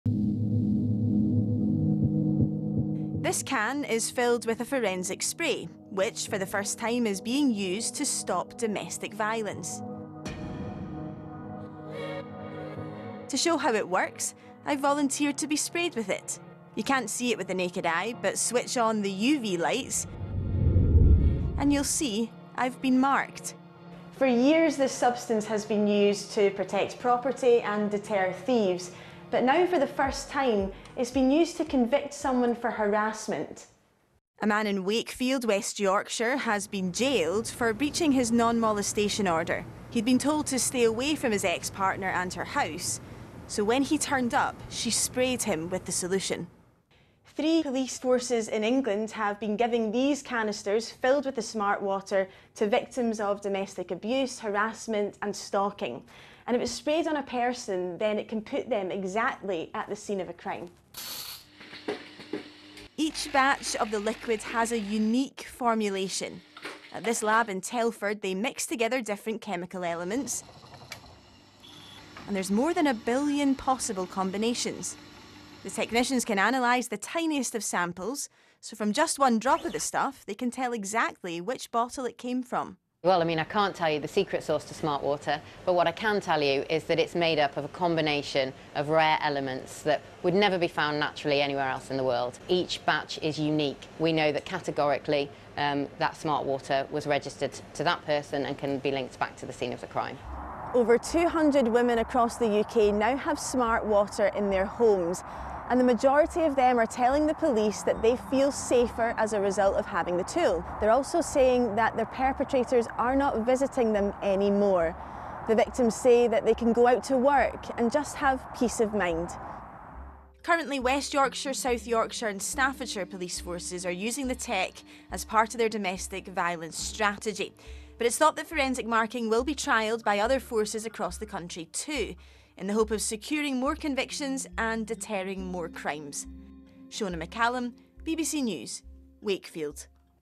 The forensic spray helping keep women safe - BBC News
The first person in the UK has been convicted and jailed for domestic abuse after being sprayed with SmartWater - a forensic liquid which shows up under ultraviolet light. The technology is being trialled by police forces in the hope of keeping women safe.

The substance stays on skin for up to six weeks and on clothing for much longer and categorically links the perpetrator to the specific batch of water that was sprayed.

Over 200 women across England now have the forensic deterrent packages in their homes. The packages include a hand-held canister for spraying, a gel for door handles and gates, and an automatic trap that sprays the liquid if someone approaches the house.